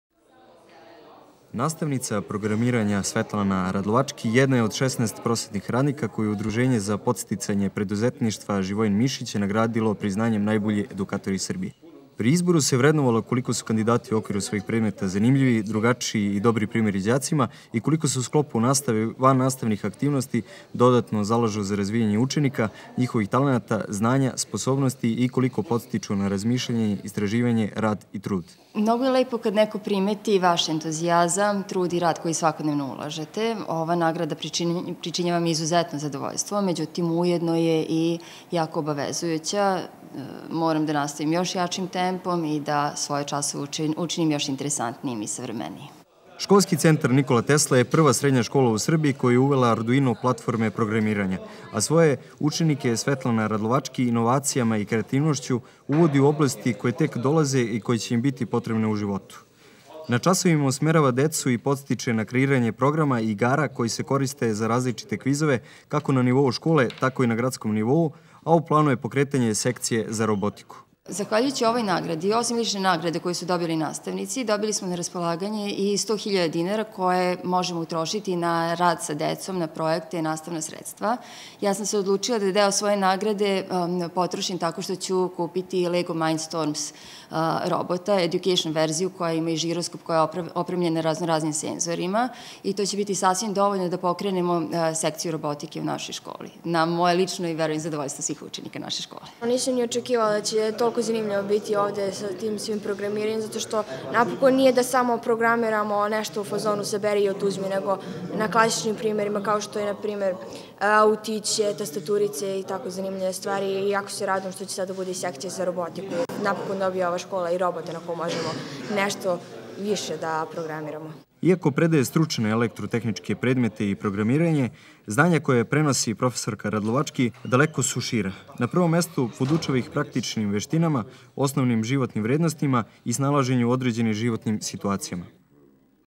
Svetlana Radlovački najbolji edukator Srbije 2015. godine
Profesorka školskog centra „Nikola Tesla“ iz Vrsca  Svetlana Radlovački dobila je priznanje za najboljeg edukatora Srbije 2015. godine. Zahvaljujući nagradi koju je dobila, u skoli u kojoj radi , kao jedna od aktivnosti, uskoro će biti pokrenuta i sekcija iz robotike.

N: Vasilije Đokić
                                          S:Igor Vojinović
Sagovornici: profesor Svetlana Radlovački
Elena Stojkovski, učenica